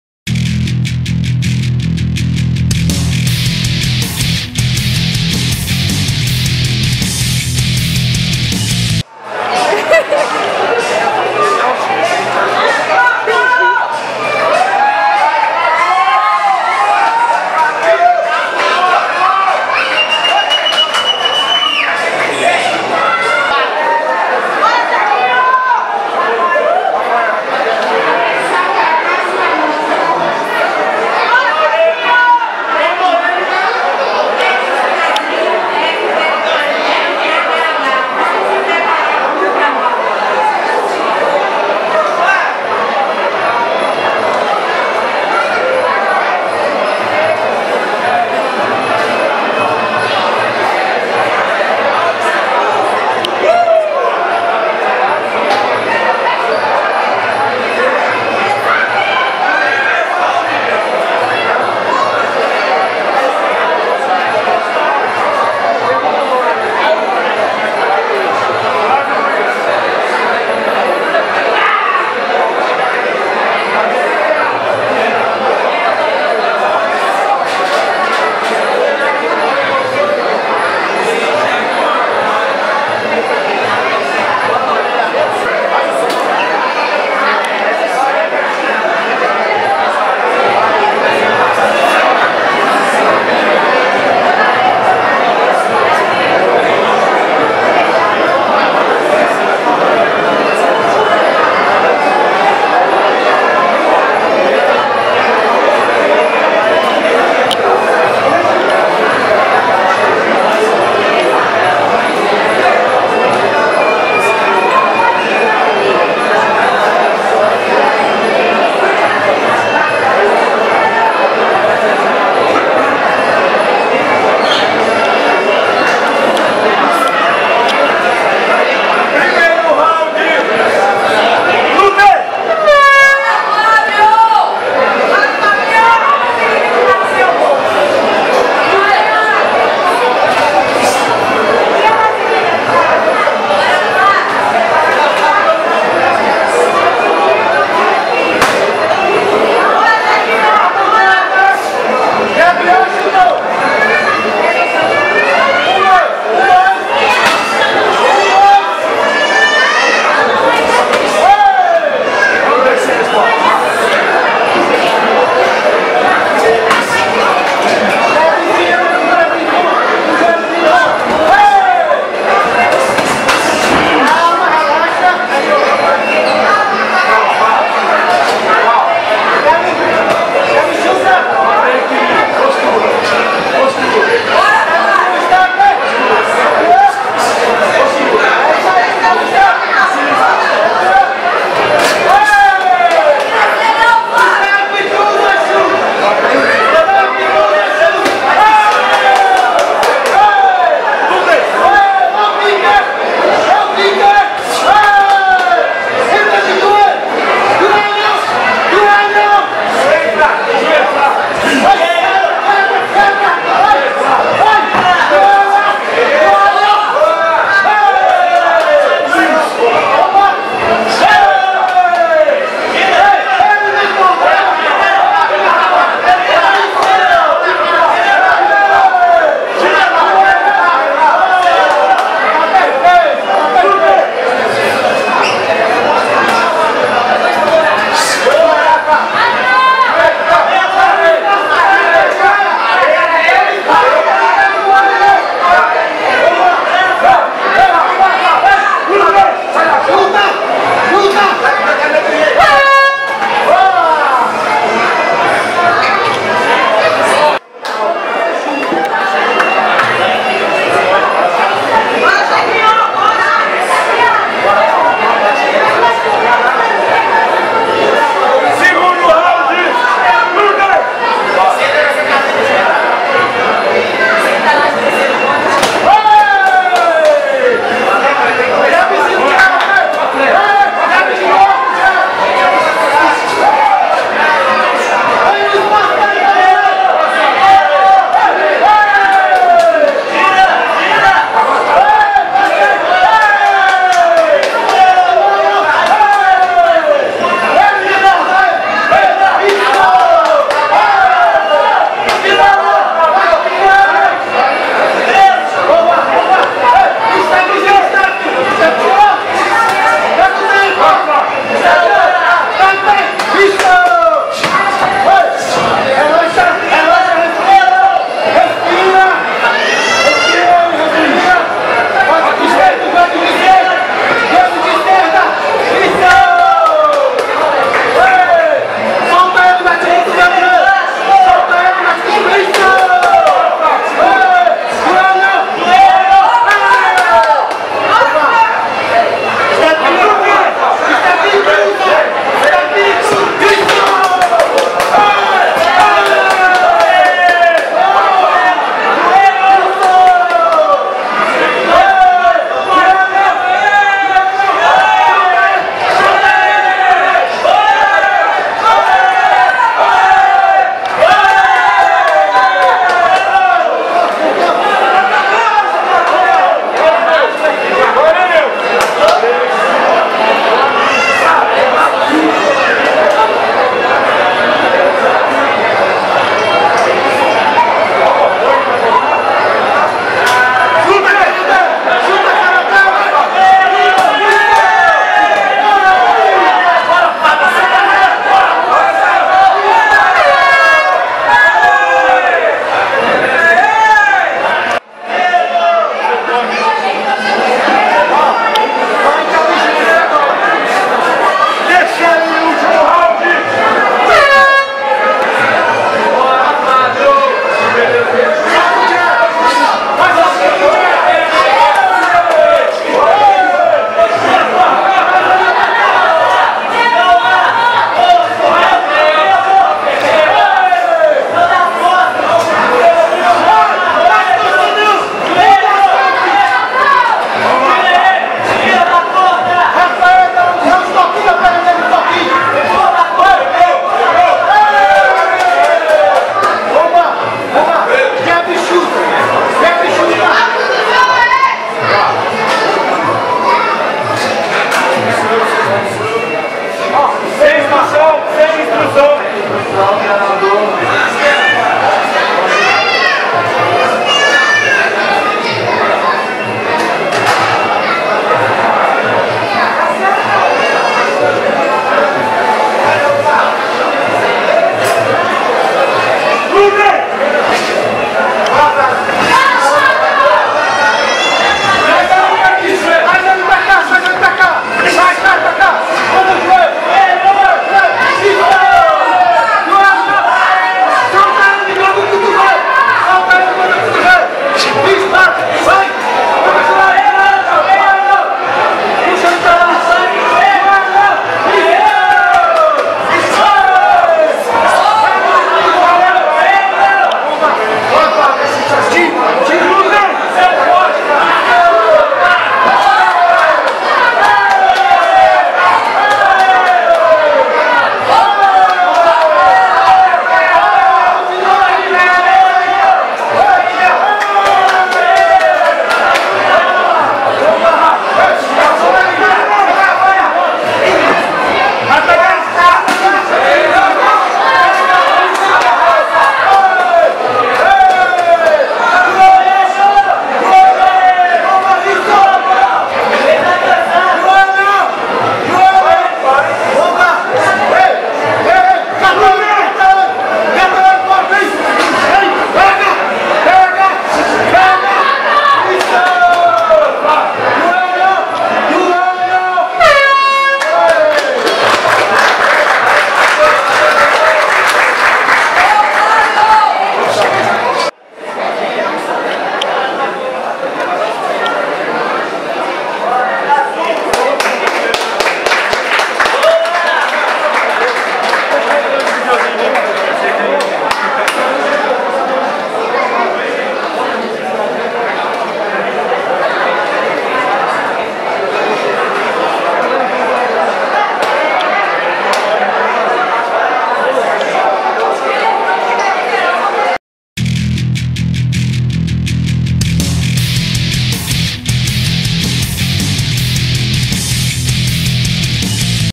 Paulo Bomba vs Flavio - Império Muay Thai 4
Paulo Bomba (União Fight) venceu Flávio Luiz (Itamar Top Family) por pontos - 63,5kg.